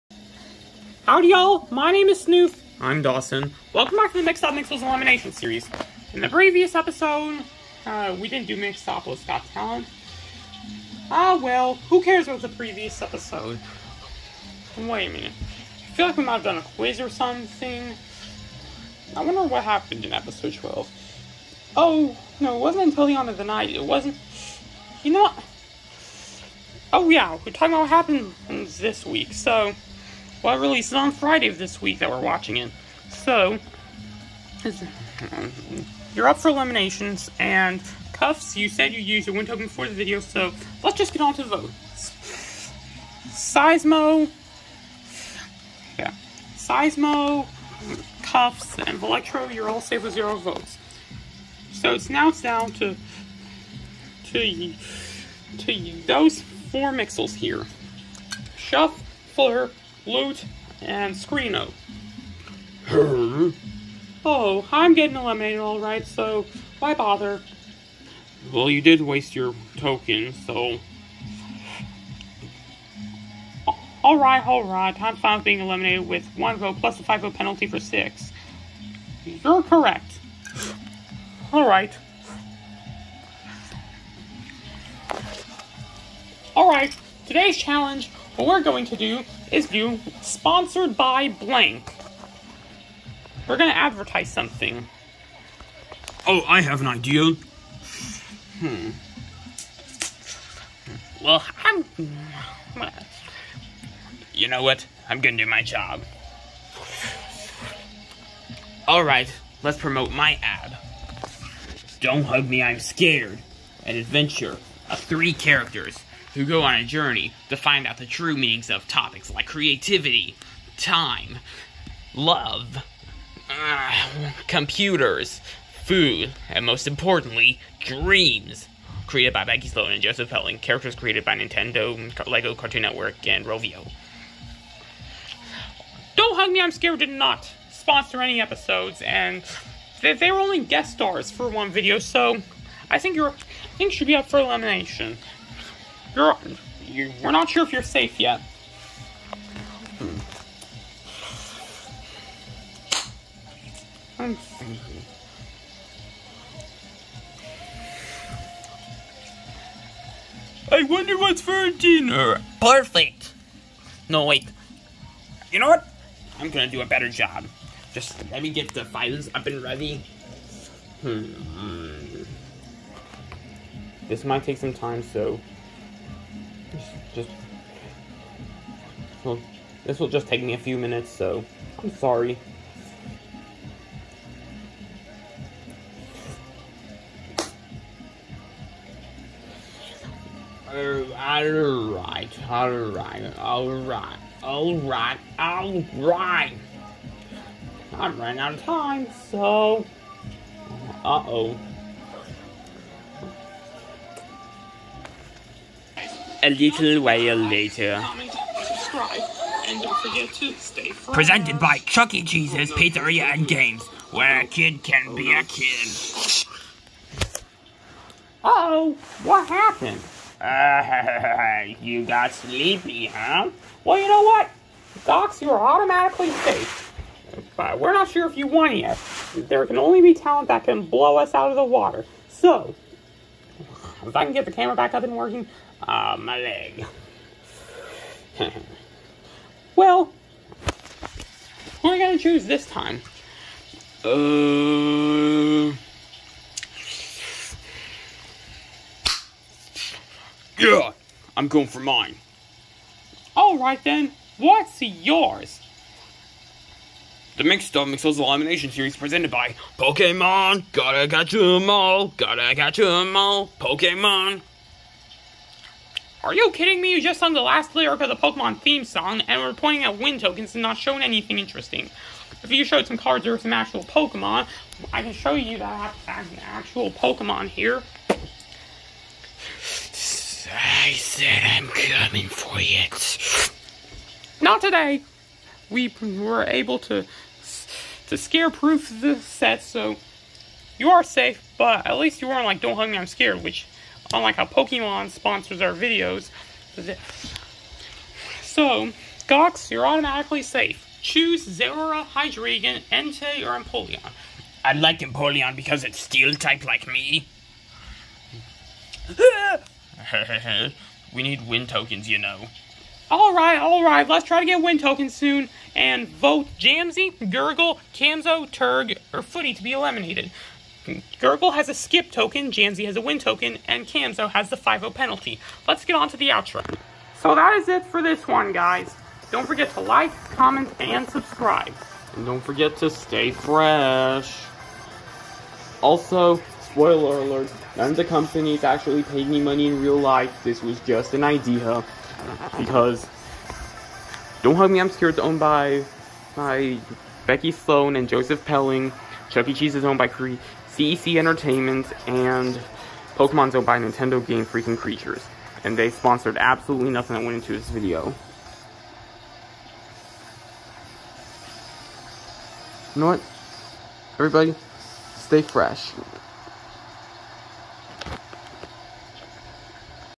The Mixed-Up Mixels Elimination Series Episode 14: "Brought to You By..."
Here's Episode 14. We advertise our sponsors with Contestants.